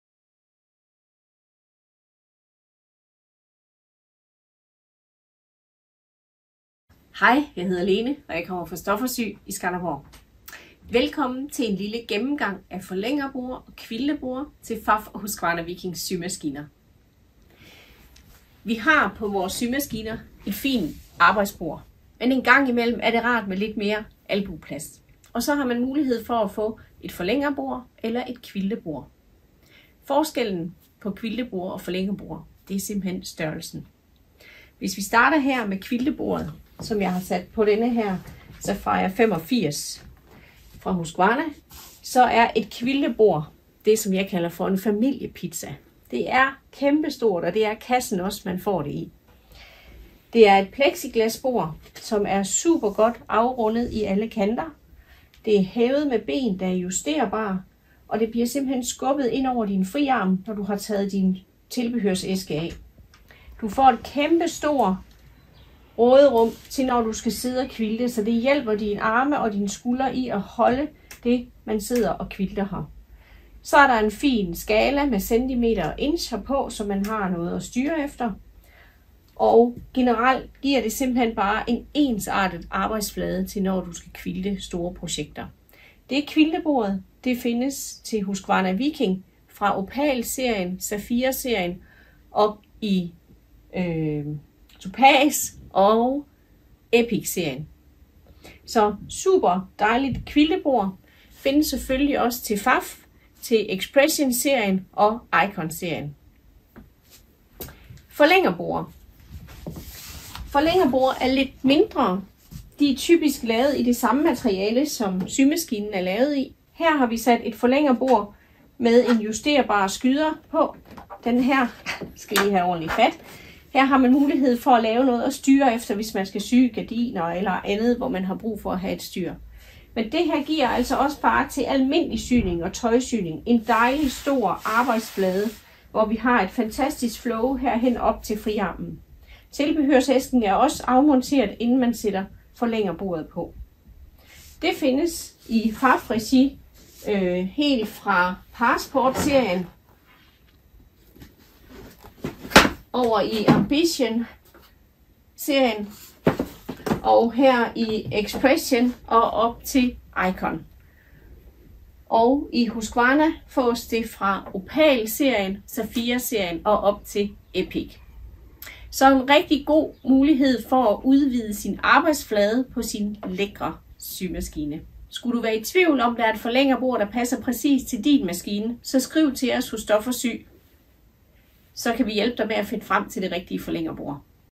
Quilte og forlængerborde til Pfaff og Husqvarna Viking
Har du brug for lidt ekstra plads, når du syr eller quilter?
Så er et  forlængerbord eller quiltebord, måske lige det du mangler.
Lene viser her de forskellige typer.